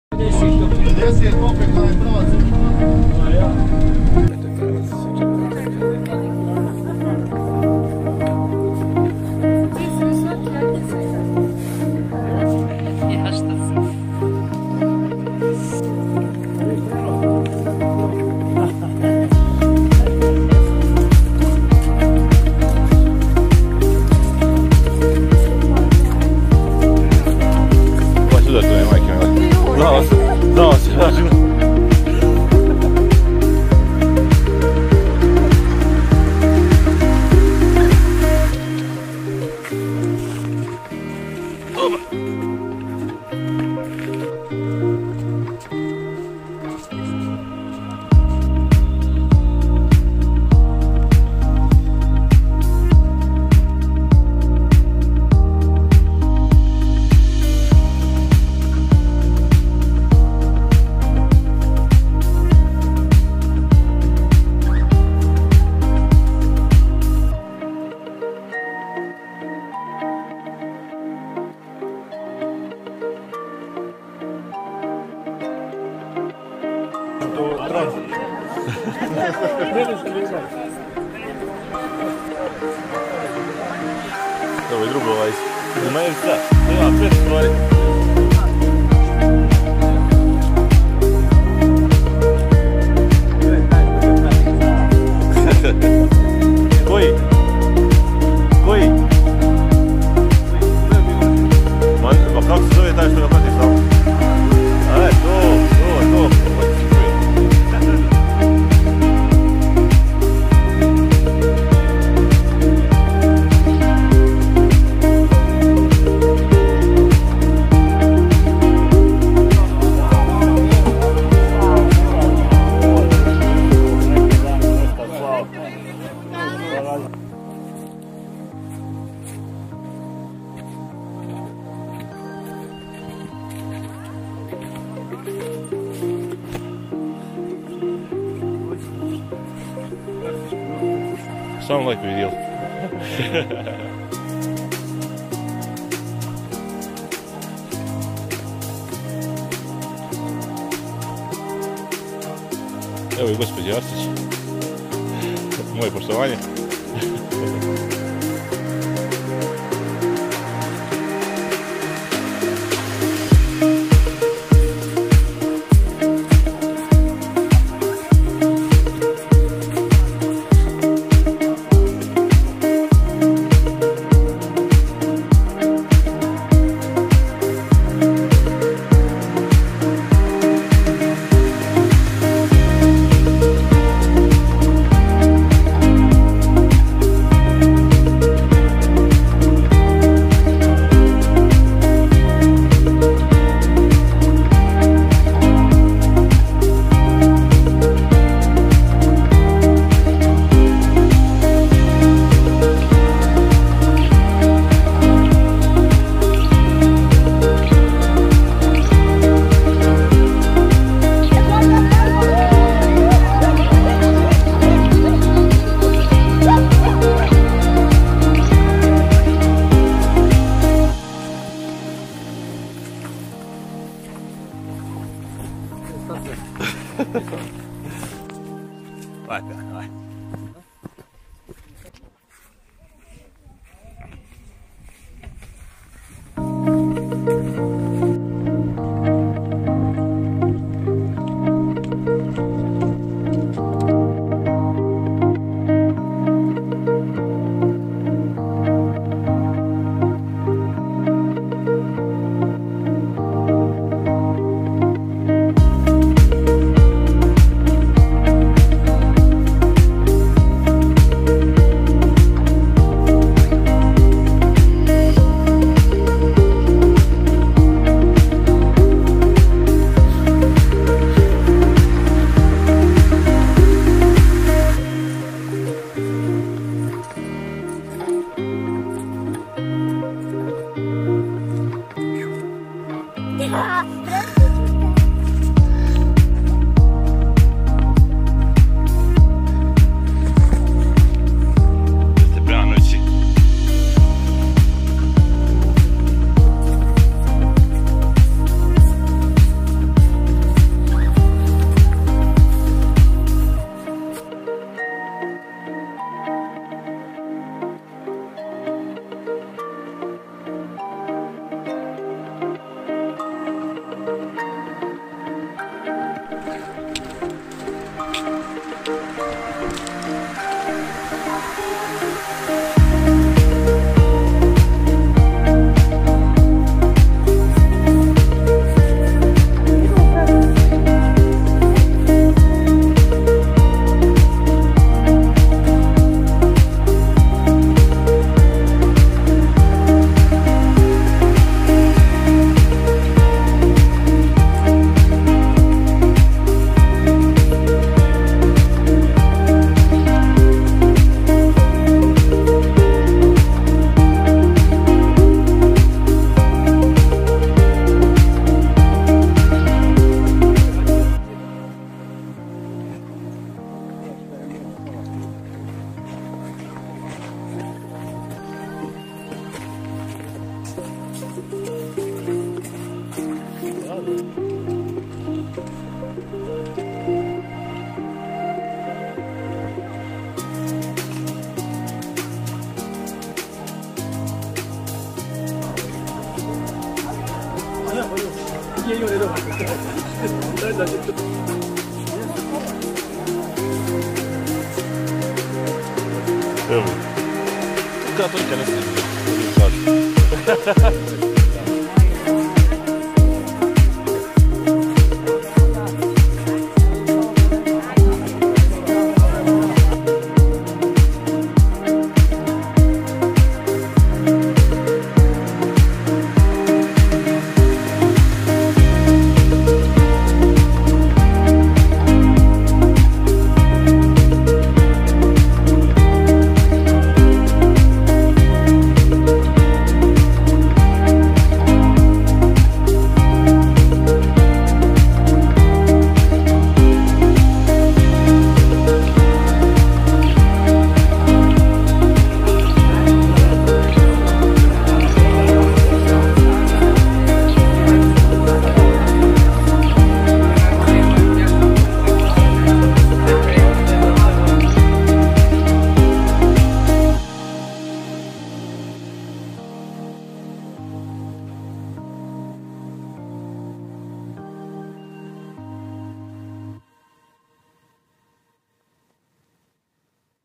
Pohod na Pjenovac 2024 | Iz ugla PED "Visočnik" Han Pijesak | SJAJAN POHOD | Gorštak
Još jedan Pjenovac. Naš uzastopni 11. Sjajne vremenske prilike i mislim nikada veći broj učesnika. Domaćini iz PED "Acer" Vlasenica ponovo na nivou. PED "Visočnik" učestvovalo sa 20-ak članova još 30-ak sugrađana od kojih su VELIKA većina učenici osnovne i srednje škole. Velika zahvalnost ŠG "Visočnik" Han Pijesak na obezbjedjivanju prevoza za naše sugradjane.
Pogledajte video na kanalu Gorštak.

Do idućeg Pjenovca - VEDRO!